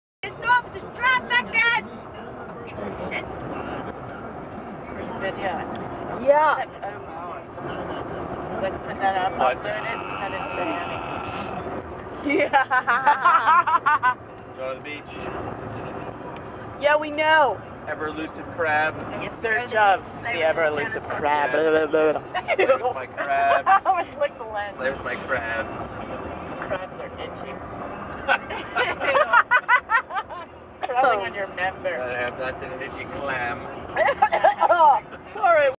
Picture 090
Drunken drive to the "beach".  8/15/09.  I, the backseat passenger and the front seat passenger were half shitfaced at this point.  Gawd knows what we were talkin bout.